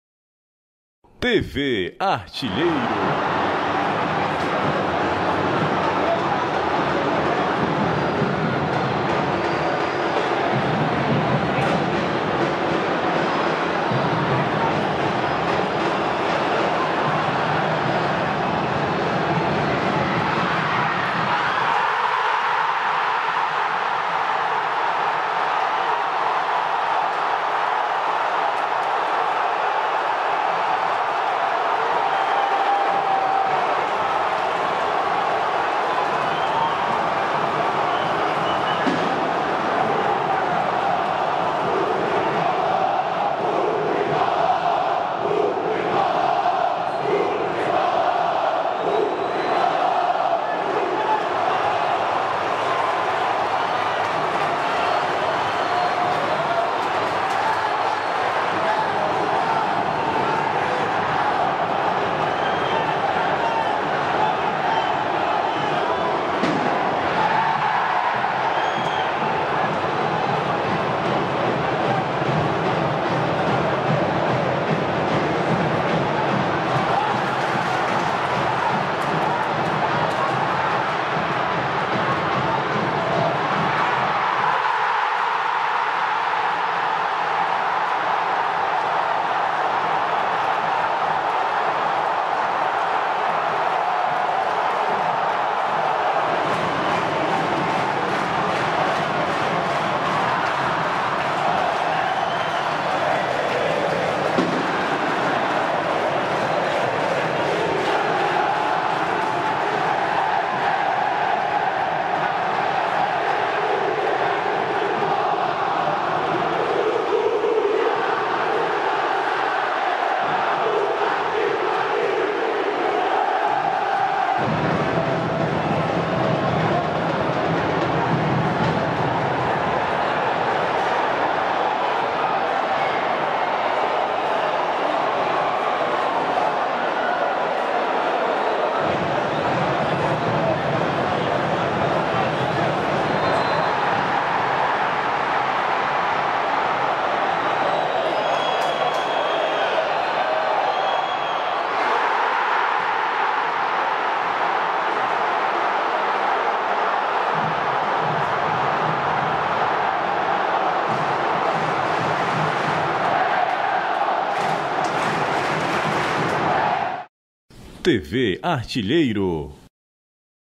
[Cearense '23] Final | Ida | Fortaleza 2 X 1 Ceará | Gols | TV ARTILHEIRO
[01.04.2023] Deu Leão! Na primeira partida da final do Campeonato Cearense, o Fortaleza venceu o Ceará pelo placar de 2 a 1. Coube os volantes Lucas Sasha e Caio Alexandre marcarem os gols tricolores e ao lateral Danilo Barcelos assinalar o gol alvinegro.